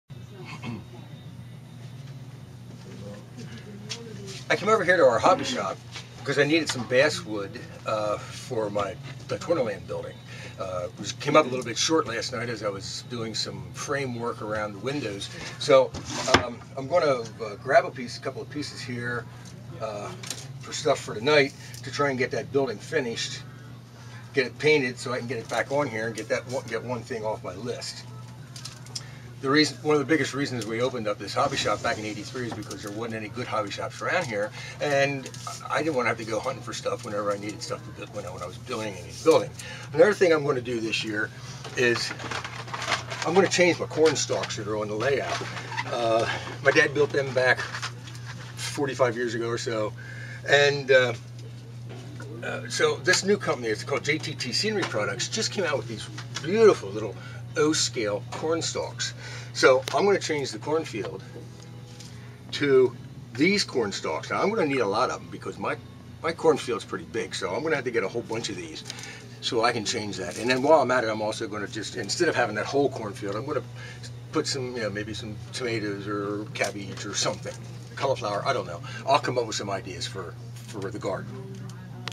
Choo Choo Barn - Shopping at the Strasburg Train Shop
Tom heads over to the Strasburg Train Shop to show us how convenient it is for him to acquire parts and pieces for the layout!
You, too, can take advantage of our hobby shop selection